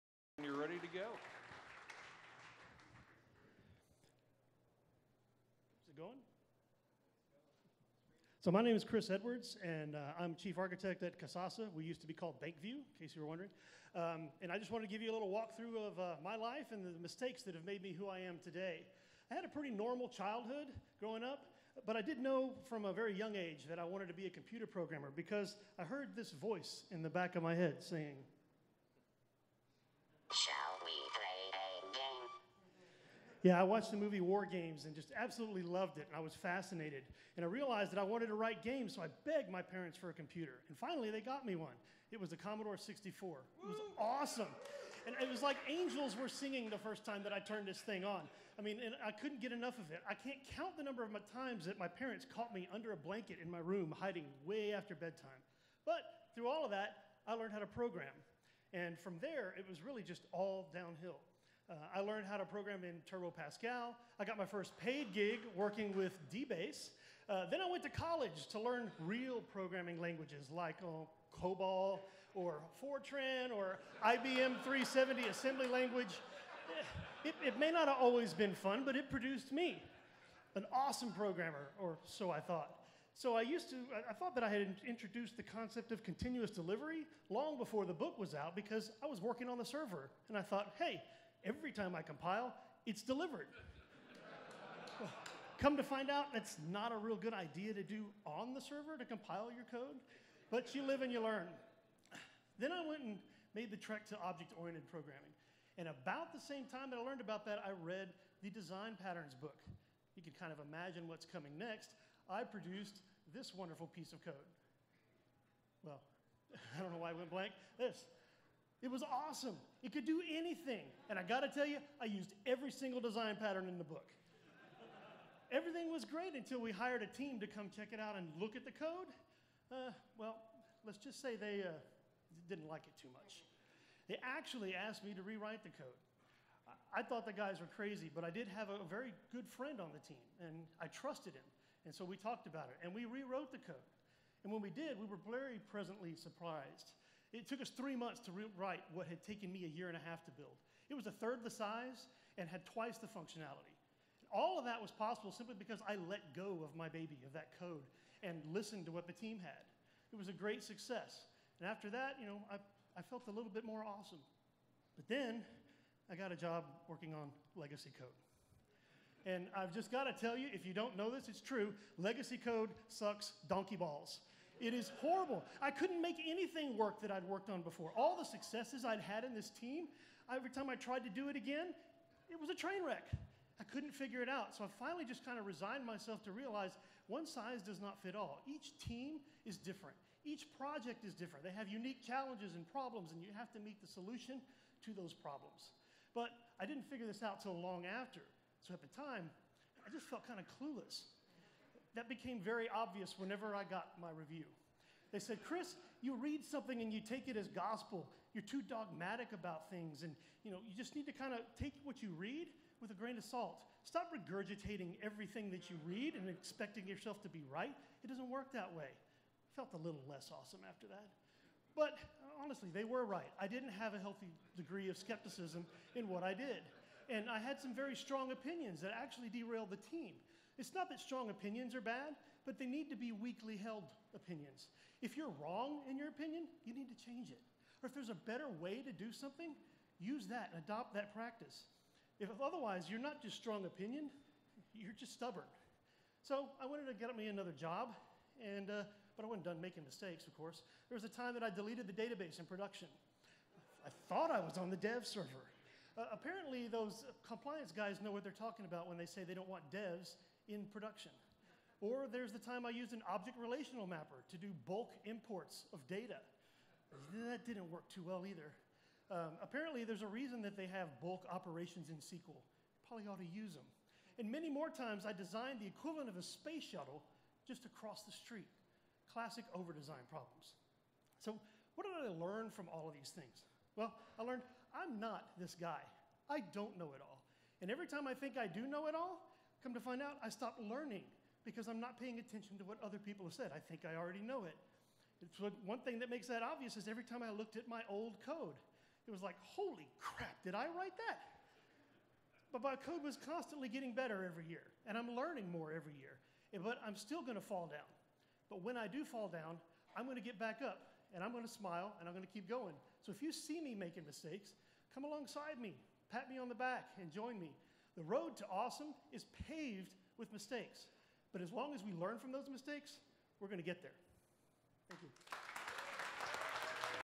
Ignite! A Developer's Progress: The mistaeks that have made me who I am
DevOps Days Austin 2016 Day 1
Speaker: Chris Edwards